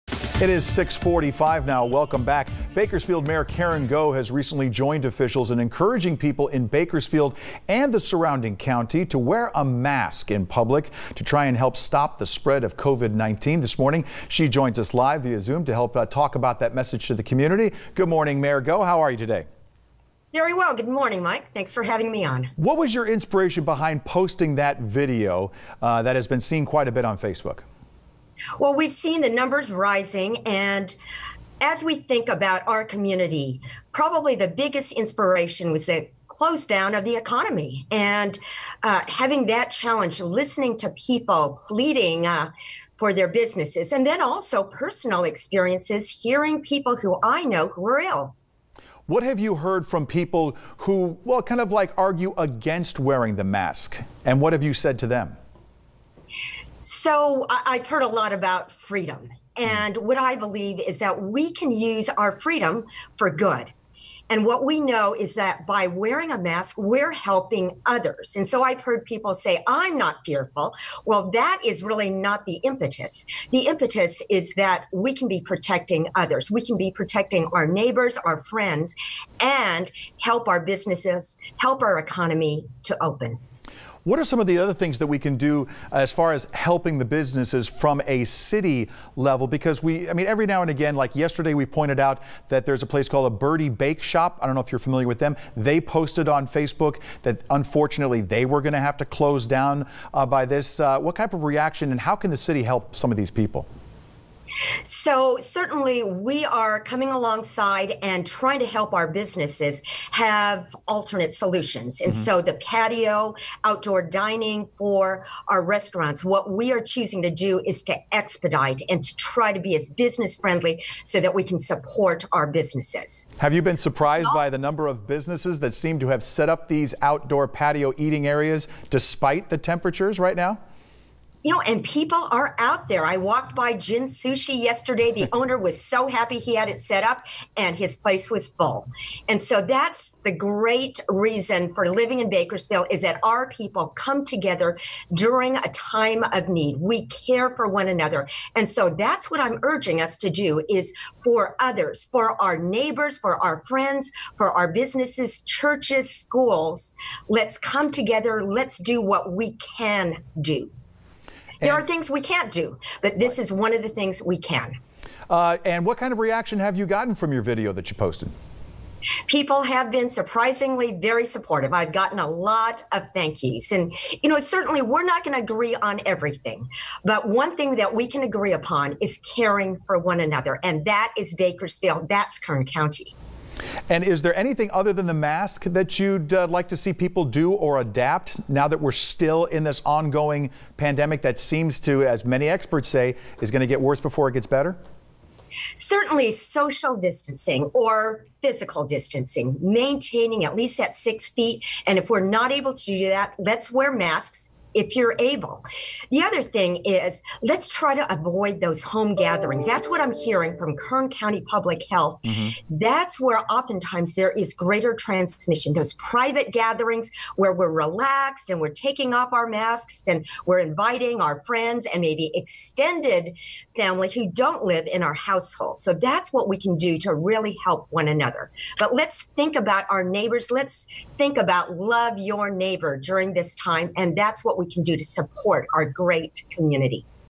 Mayor Karen Goh on masks
Mayor Karen Goh encouraging the public to wear masks